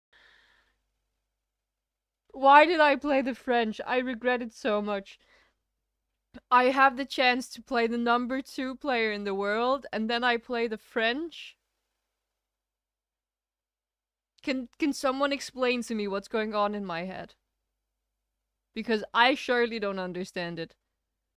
PlAyInG tHe FrEnCh AgAiNsT WORLD #2, GM Ding Liren
I had the chance of playing the number two player in the world - and what do I do? Play the French! This choice was quite quickly met by a feeling of regret...